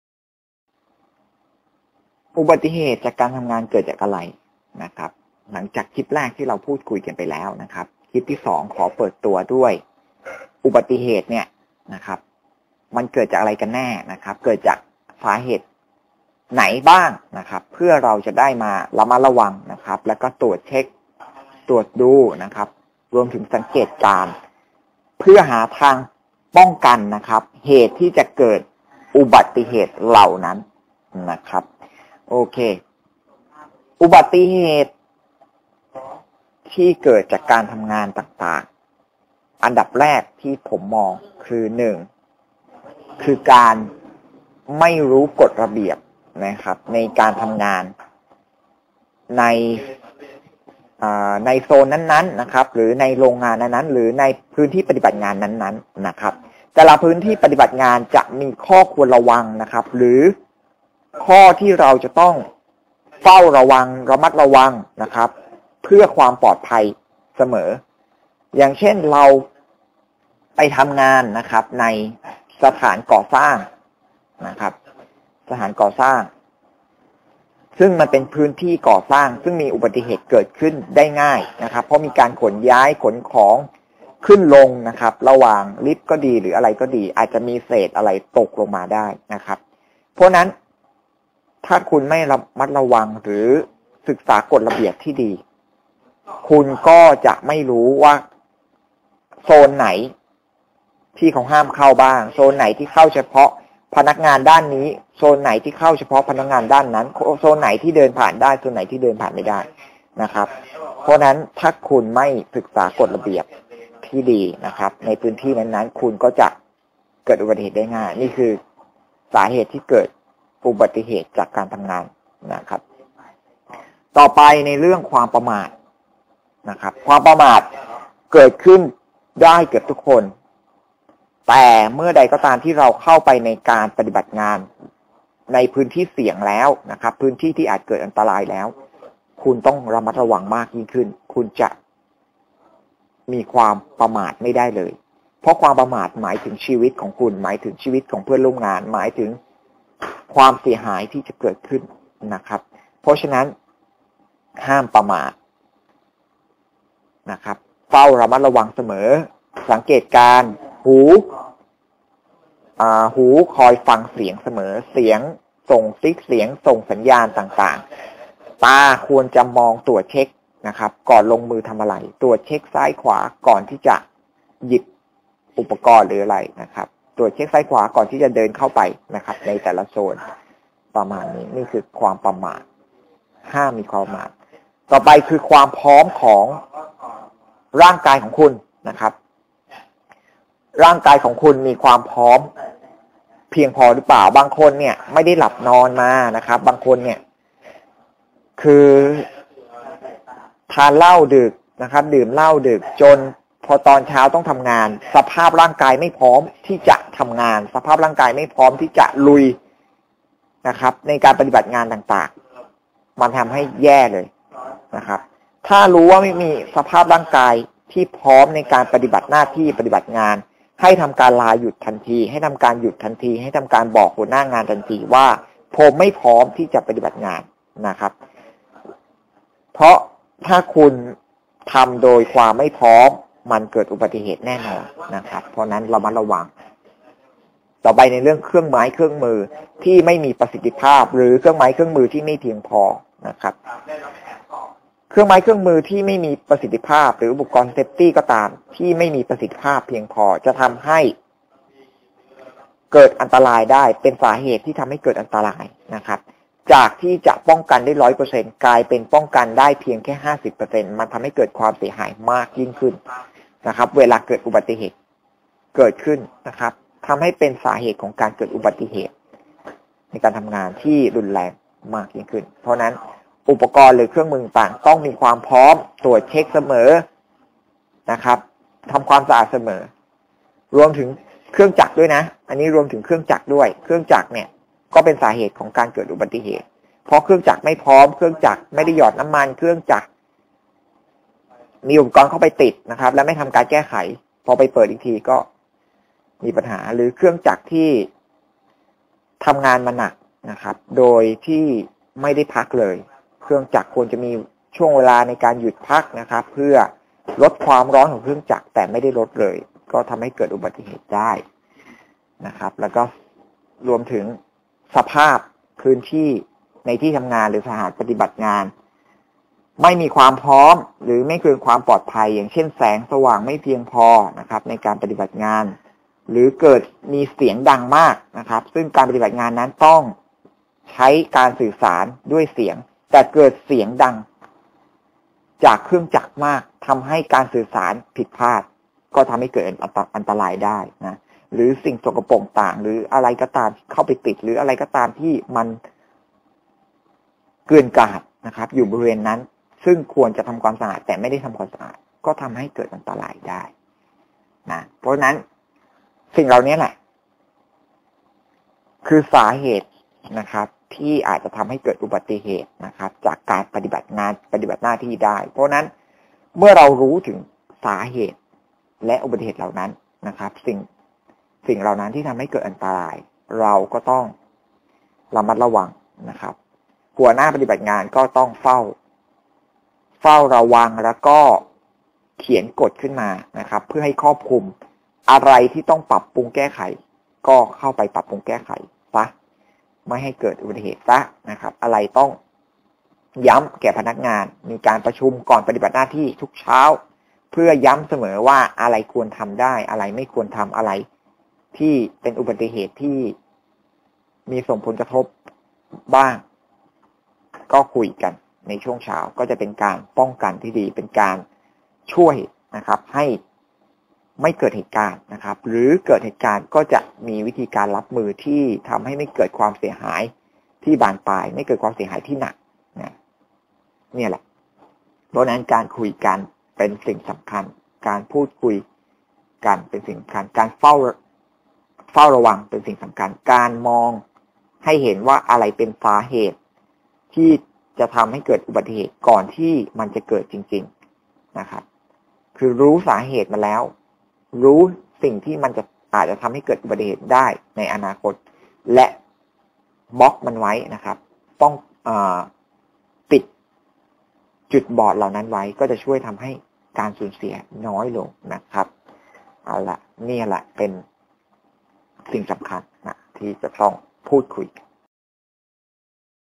ความปลอดภัยในการทำงาน ep2 อุบัติเหตุเกิดจากอะไร มาเรียนรู้และหาทางป้องกัน
อุบัติเหตุเกิดจากอะไร มาเรียนรู้และหาทางป้องกัน

สนับสนุนความปลอดภัย โดย เว็บไซต์ จัดจำหน่ายอุปกรณ์เซฟตี้ อุปกรณ์เพื่อความปลอดภัย